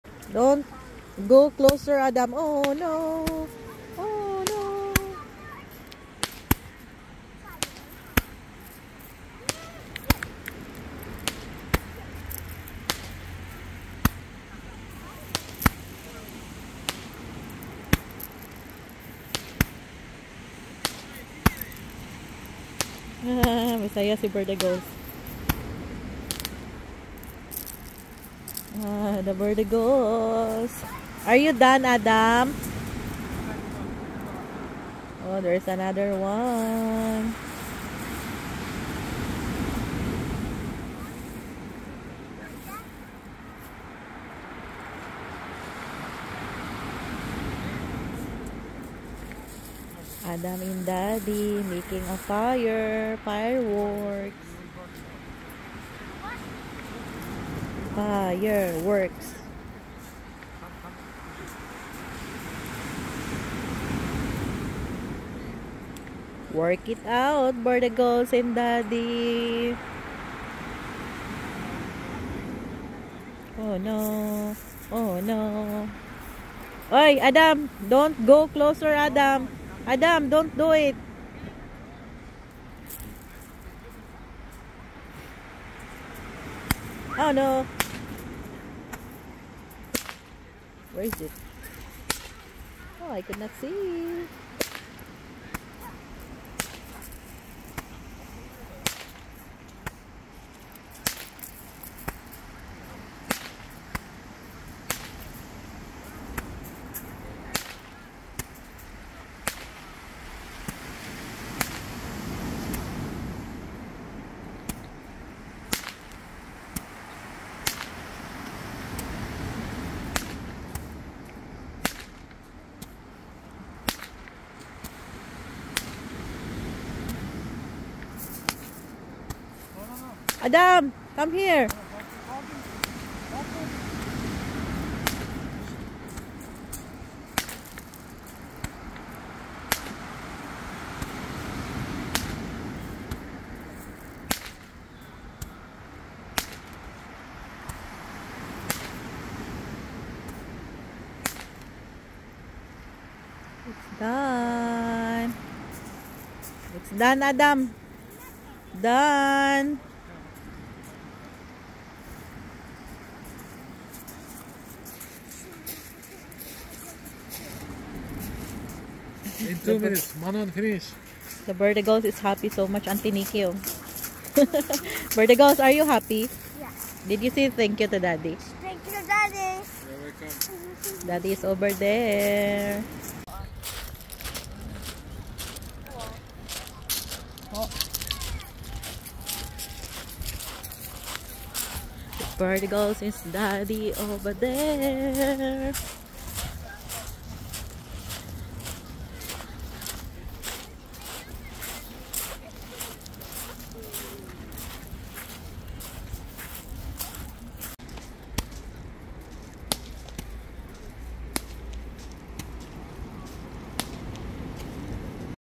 adam and his daddy set off a fireworks
Ulsan camping site sa tabi ng dagat